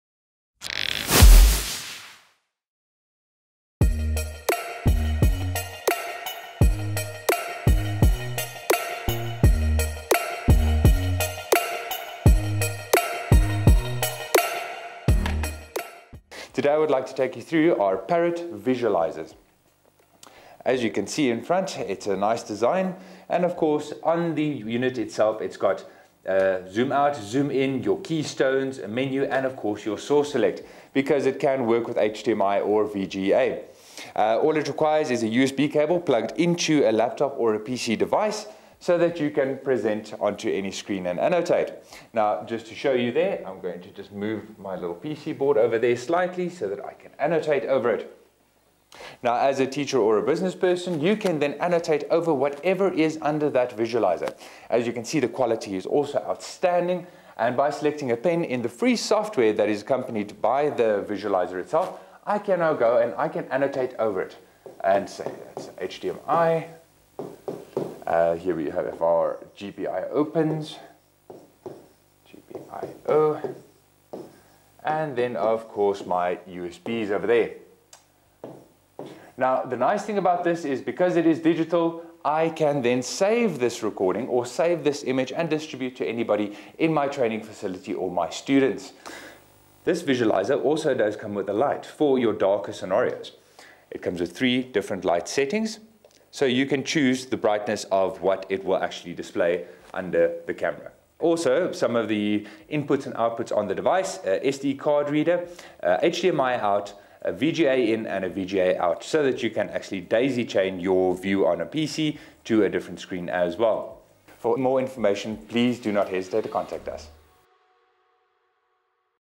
Parrot Products (Pty) Ltd - Using the VZ0002 visualizer with an Interactive LED Touch Panel
In this video, we will show how you can use a VZ0002 visualizer camera with an Interactive LED Touch Panel which enables you to turn a physical object into a digital representation and allowing you to annotate over it and send it to pupils or work employees.